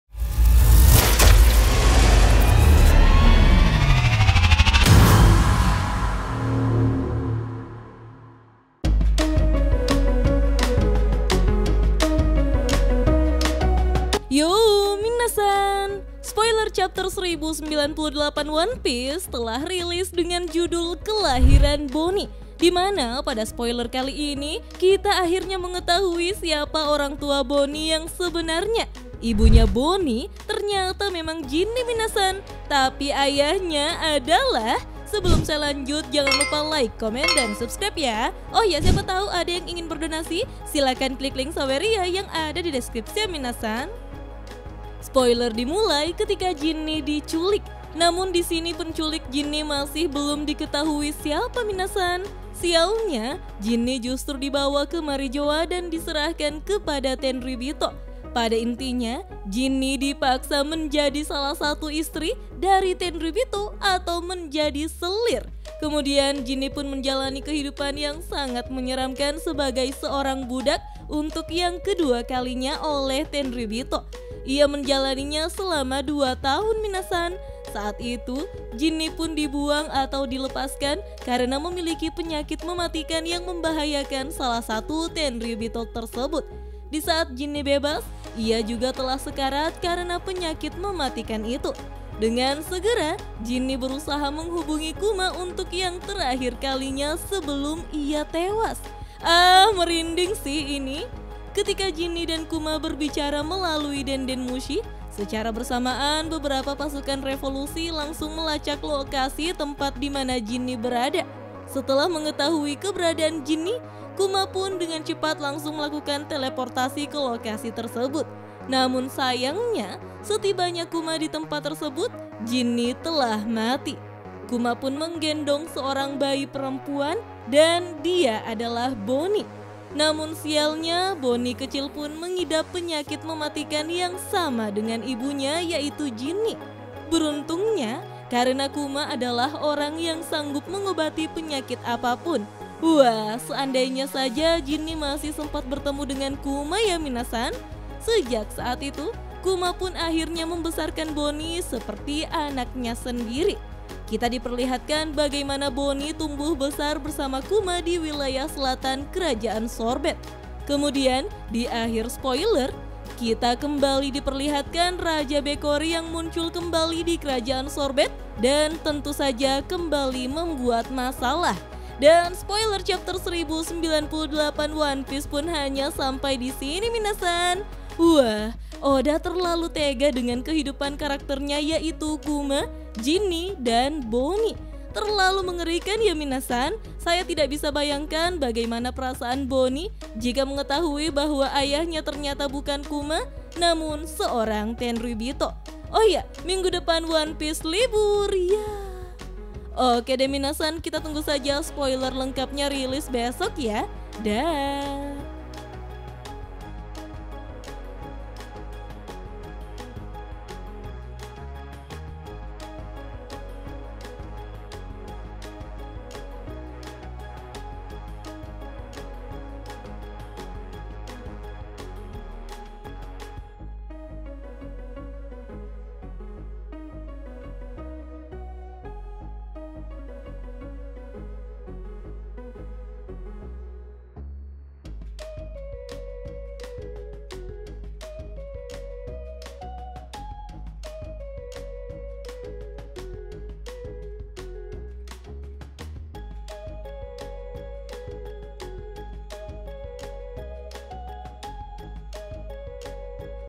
Spoiler Chapter 1098 One Piece - Ayah Bonney Bukan Kuma - Tapi Salah Satu Tenryubito!
spoiler Chapter 1098 One Piece telah rilis dengan kelahiran Bonney, di mana pada chapter kali ini kita akhirnya tahu siapa ayah Bonney yang sebenarnyanya. Bukan Kuma, melainkan salah satu Tenryubtio!

Buat Minna-san yang ingin donasi untuk channel ini bisa klik di sini